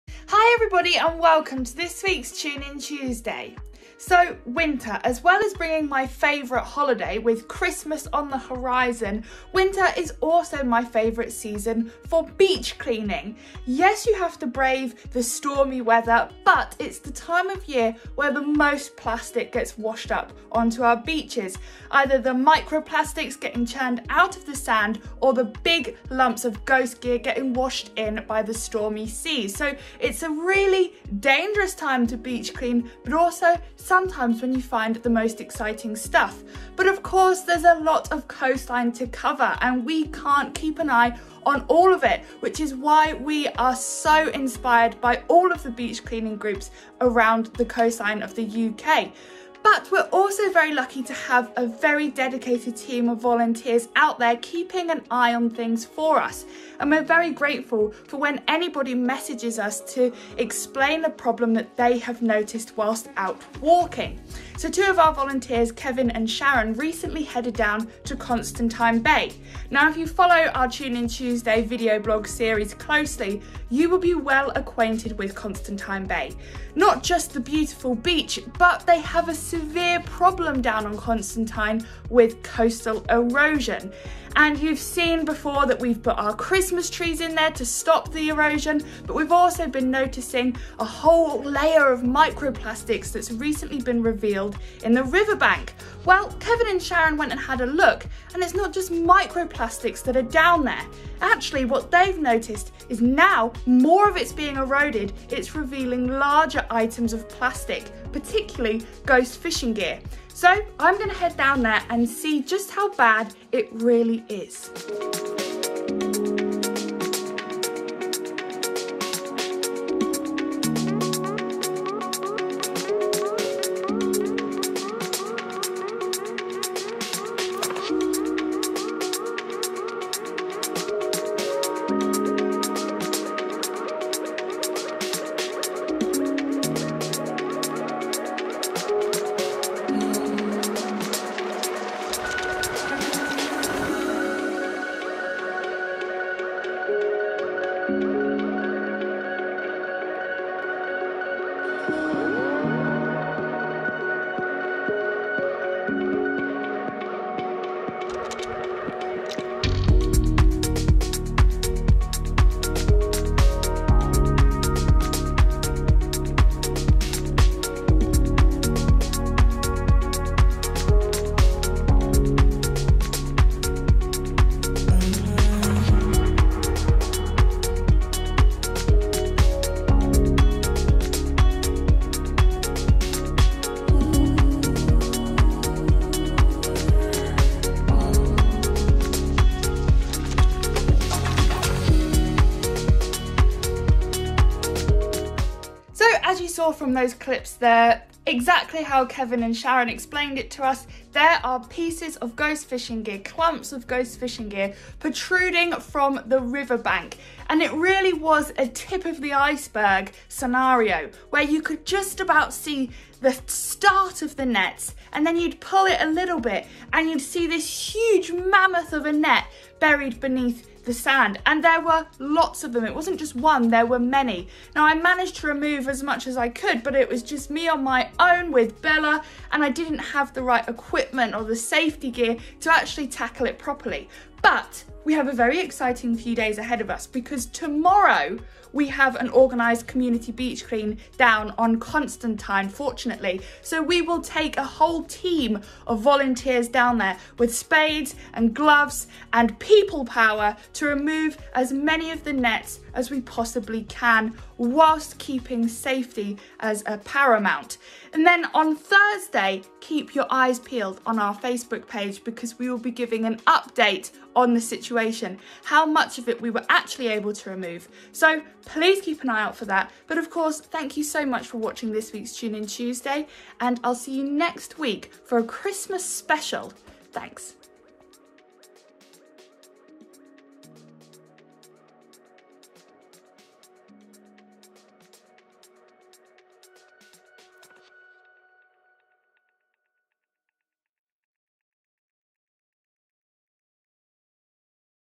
So much GHOST FISHING GEAR on Cornwall beach clean up VLOG!
TuneInTuesday Episode 124! 🌊 Winter! It brings stormy seas and a fresh wave of waste! This time of year is where we get to see some of the most extreme beach cleaning and plastic pollution clean ups here in North Cornwall UK; and this beach clean up down on Constantine Bay was certainly no different! As the sand dunes move and change due to coastal erosion, old forgotten plastics and litter is reveal. In this case, bundles of old ghost fishing nets; formerly buried under the sand, but now revealing themselves once again, which of course presents a serious hazard to wildlife and marine life, and could end up back in the ocean! So, check out exactly what Beach Guardian's Emily Stevenson found on her beach clean up at Constantine Bay, Cornwall! Would you like to see more beach clean videos like this one?